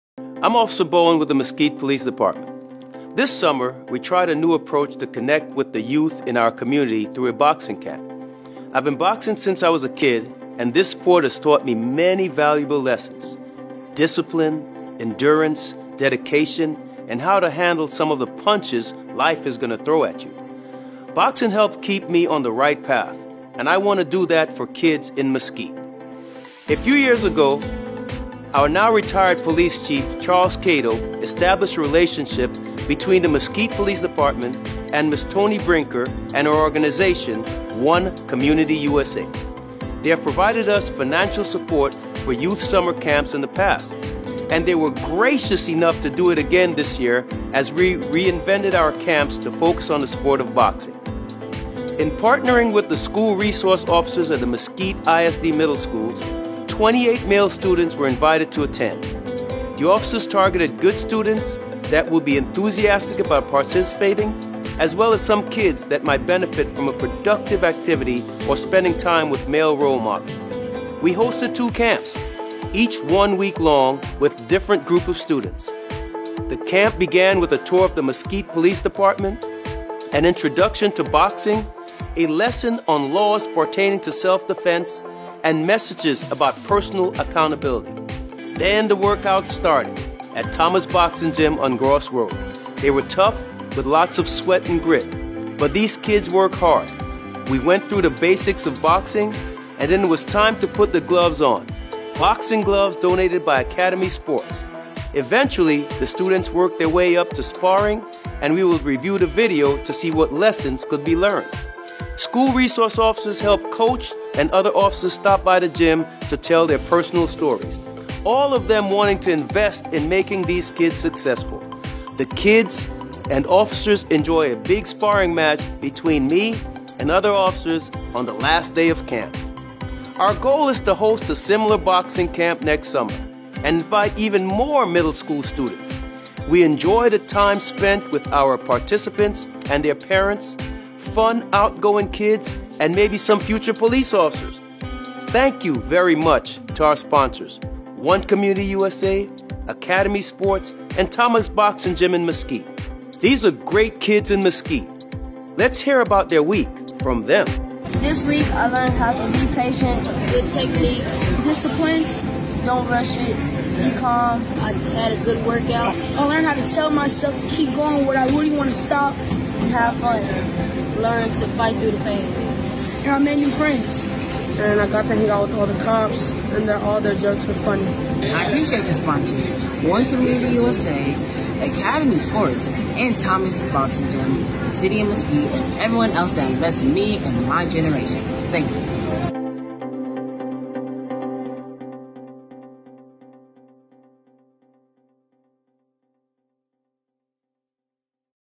Boxing Camp at Mesquite ISD '21
The Mesquite Police Department hosted a Boxing Camp (in conjunction with One CommunityUSA) in the month of July and it taught many valuable lessons to some Mesquite ISD students.